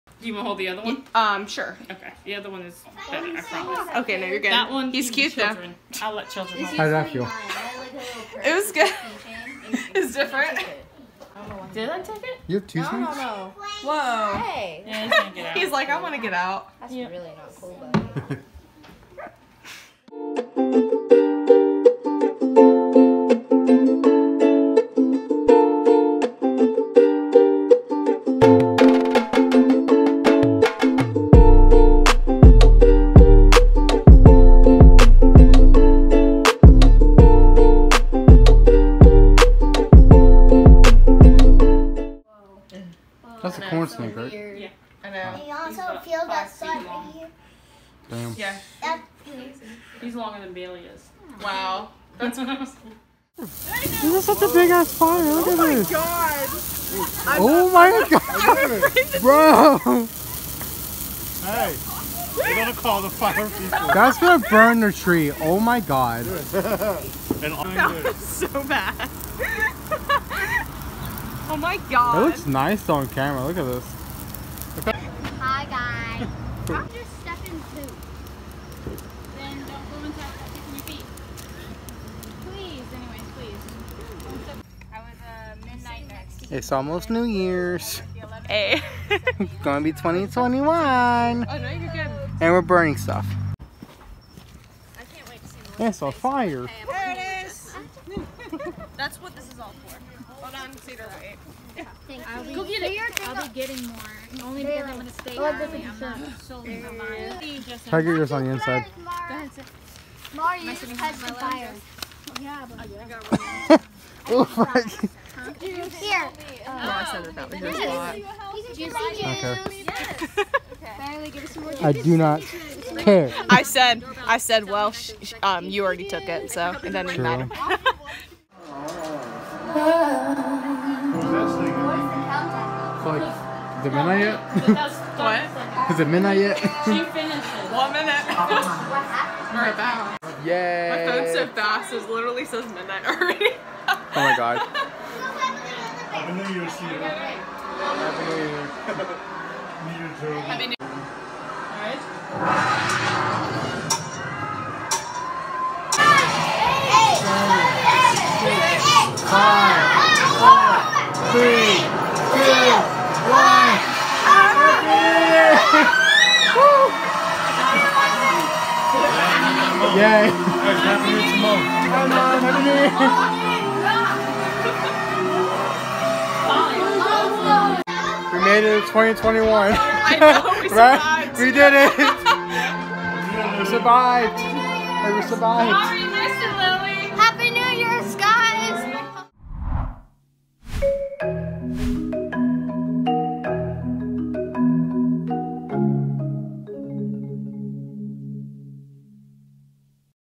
A new year (2021)
Celebrating the day 2020 goes to 2021, the end of a decade and into a new one. I hope the best for everyone and I hope this year is a turnaround and things get better!
Had my best friend, sisters and family around me. Happy new year y’all